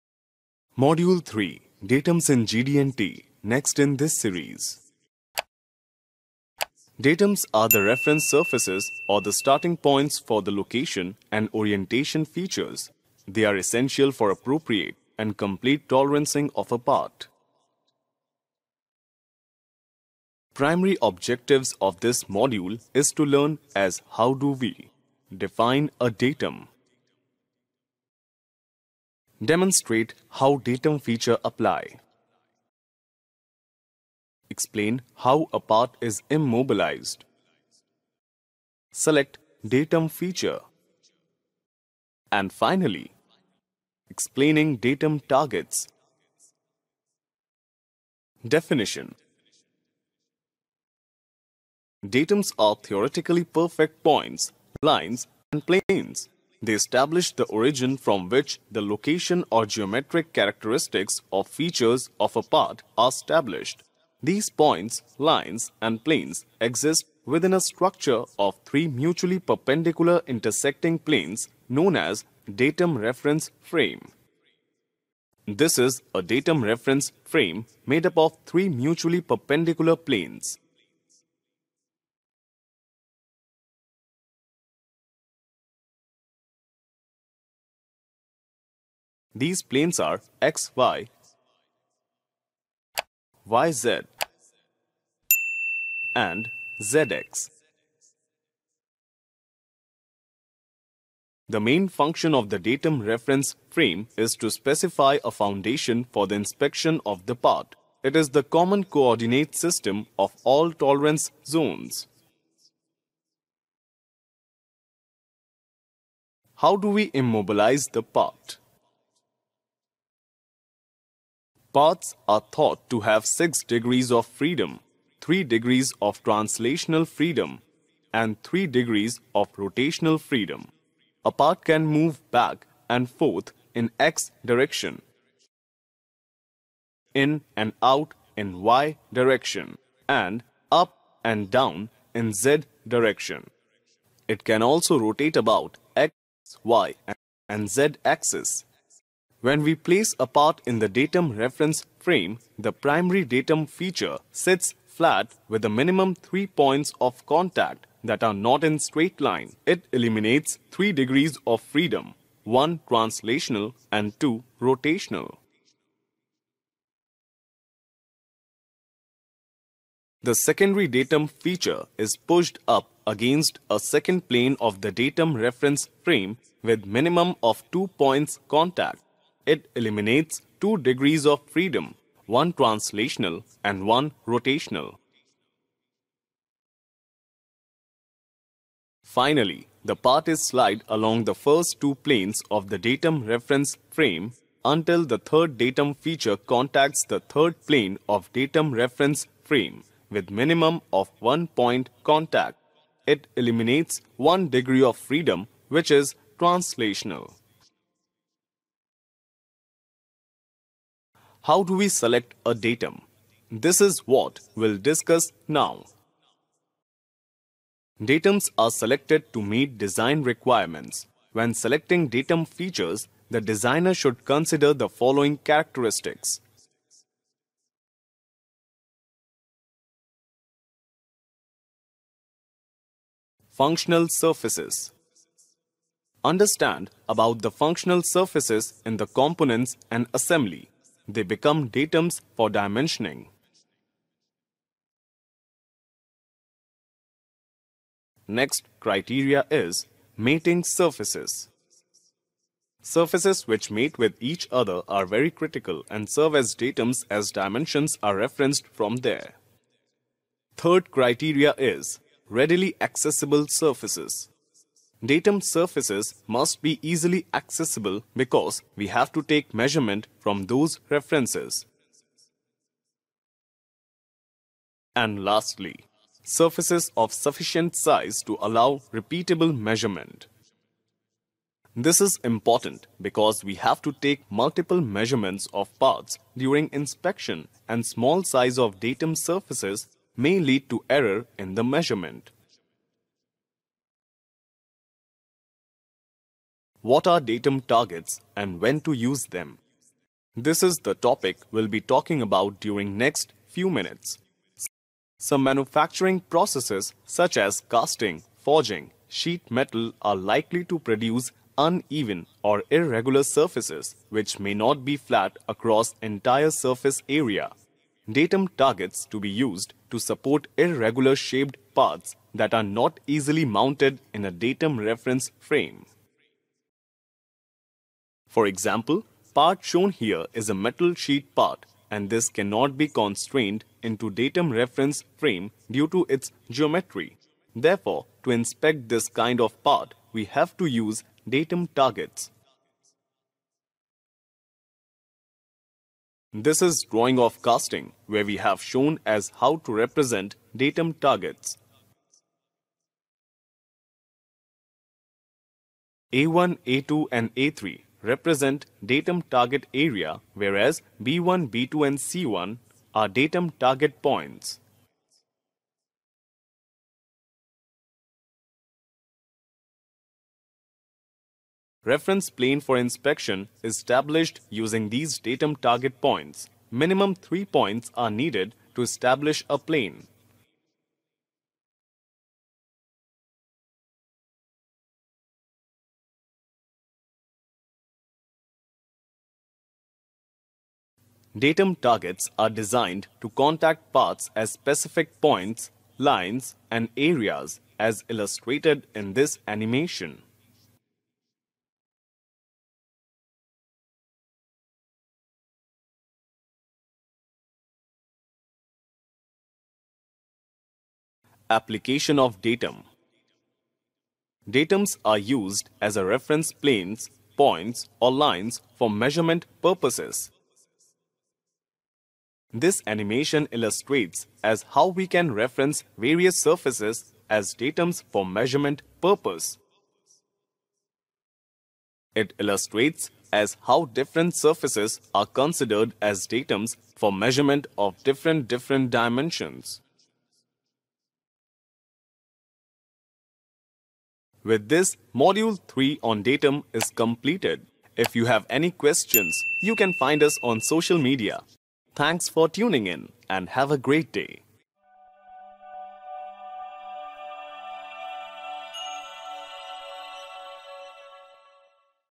Datums in GD&T
This videos explains all about Datums, their function and how are they selected and what's their role in establishing GD&T tolerancing.

This video was created for beginners to quickly understand, interpret and implement all the concepts of Datums while using GD&T in mechanical drawings.